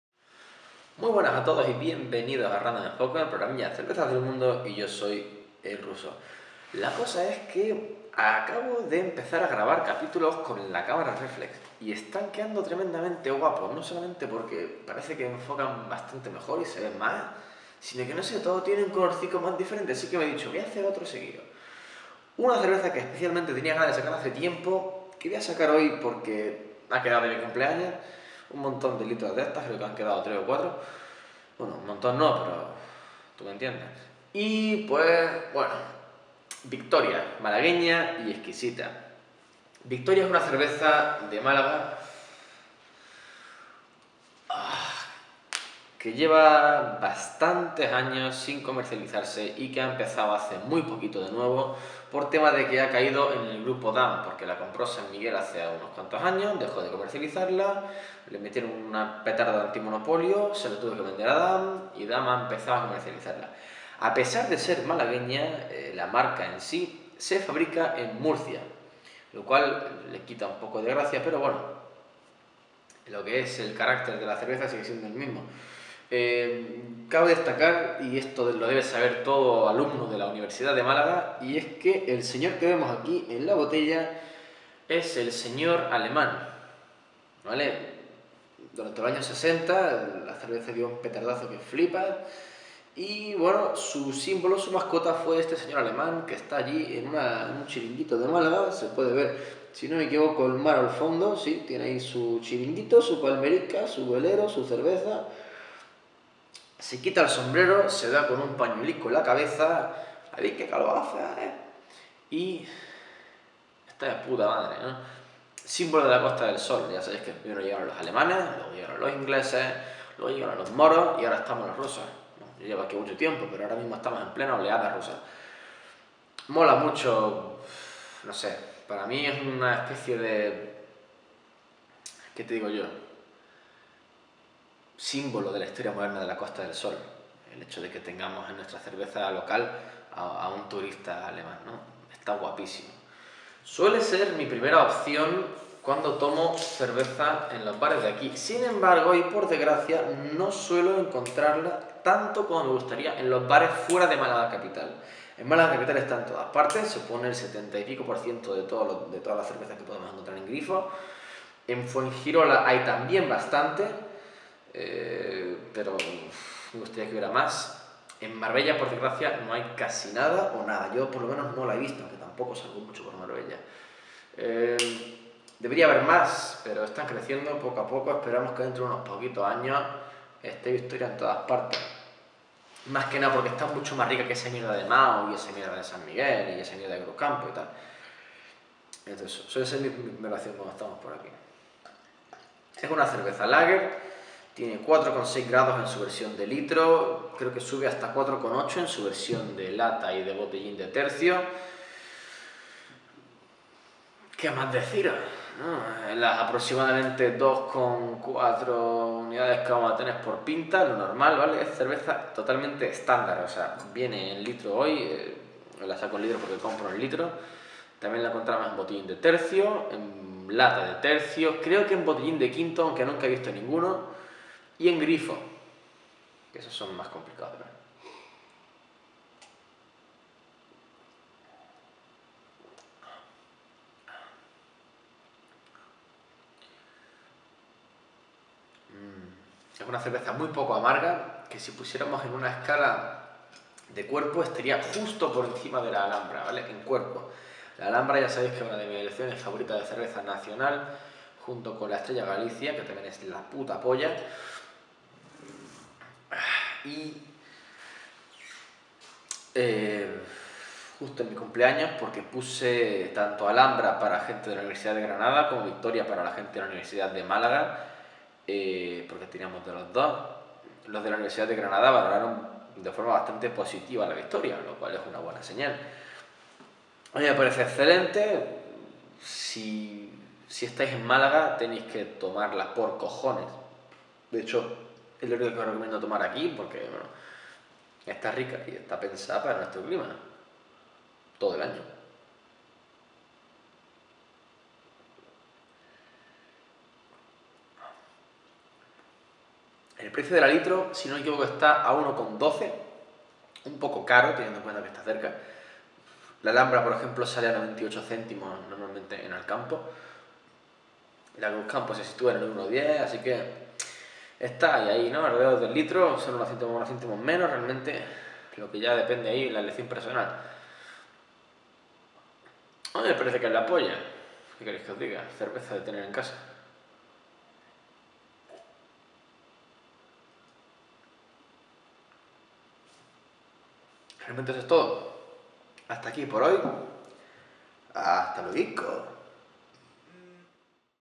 MALAGUEÑA Y EXQUISITA! CdM - 44 Victoria
Cerveza Lager de Málaga (aun que se fabrica en Murcia)... Todo un icono...